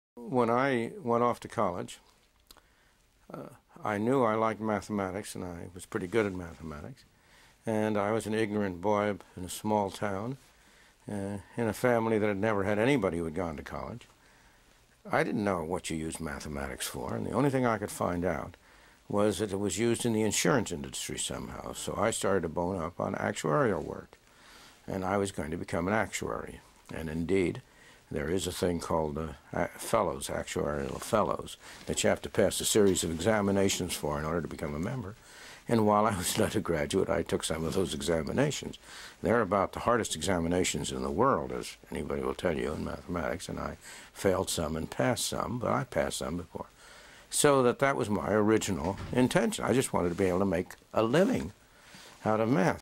Milton Friedman wanted to become an Actuary
Apparently The Nobel Laureate wanted to become an actuary but quickly realized how tough the exams are.

I do not take any credit for this video as it was freely available for download at: